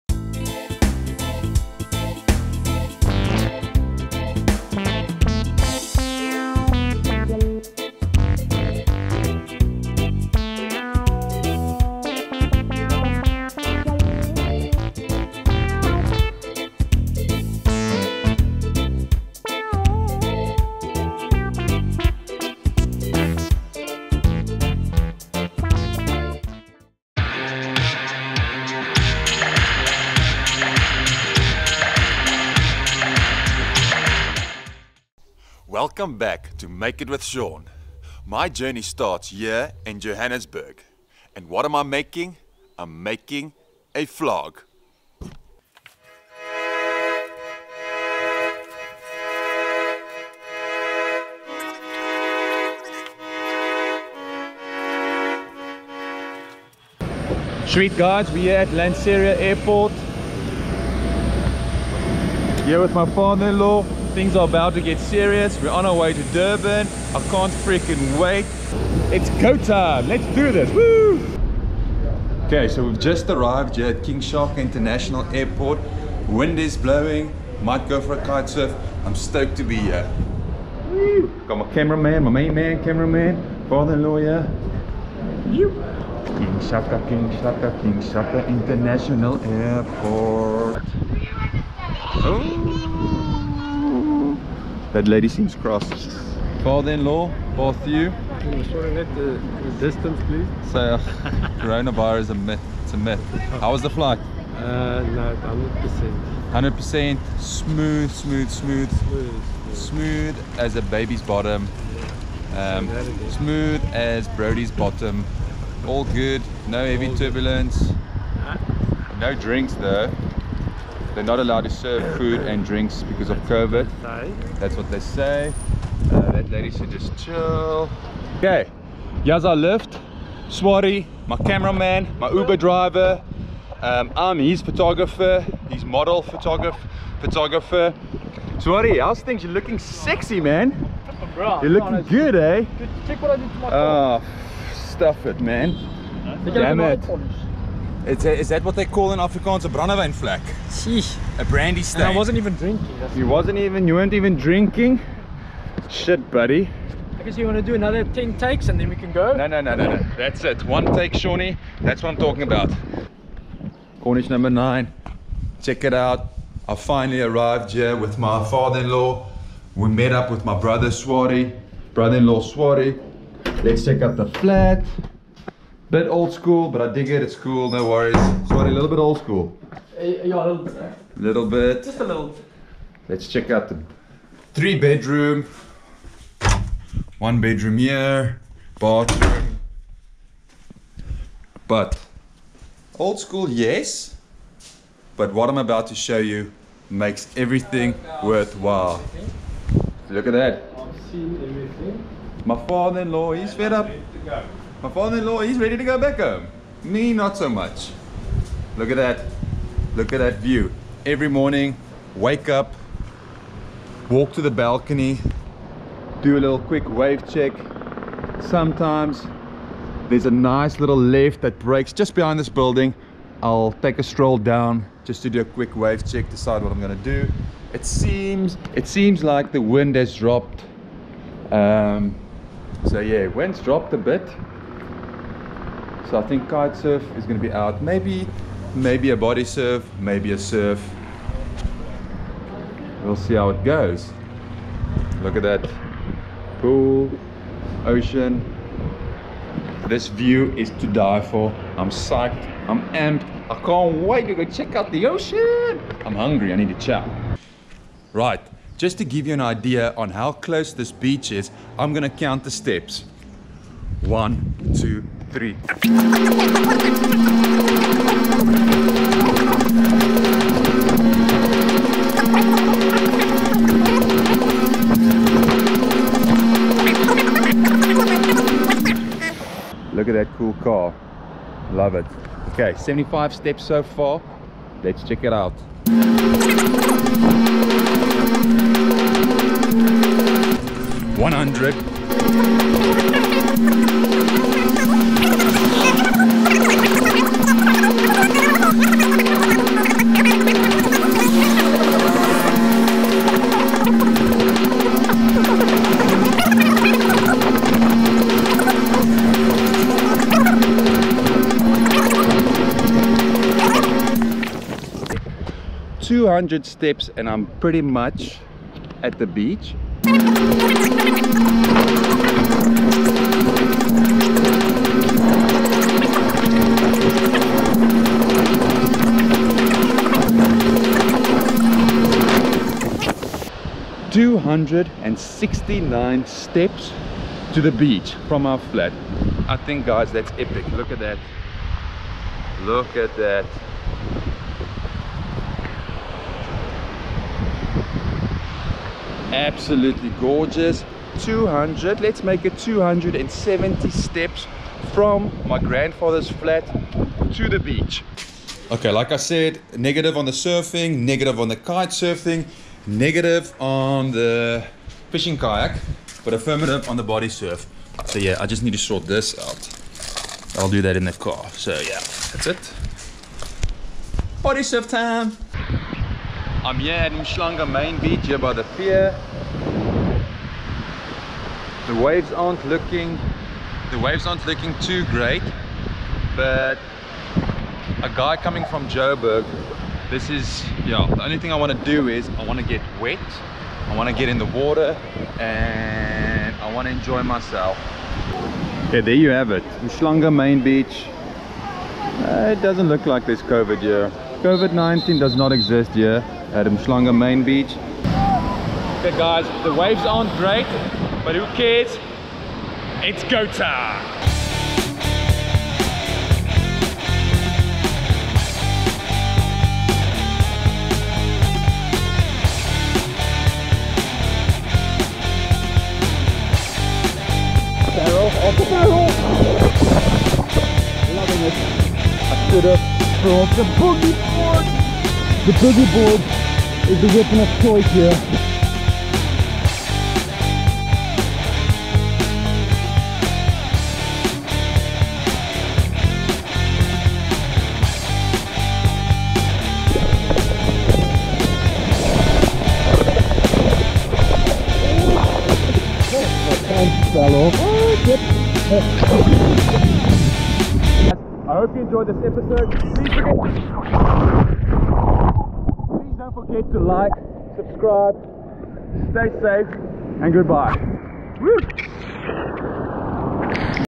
Make it with Shaun. (MIWS)TRAVEL VLOG (Umhlanga ep 1) Be creative and let's make it!
First day in Umhlanga (Durban).  South Africa. I'm making a travel vlog. And enjoying life!
I'm on a mission to find waves, fish or wind...
I decide to go for a body surf and caught some fun little waves. Umhlanga Rocks is the place to be!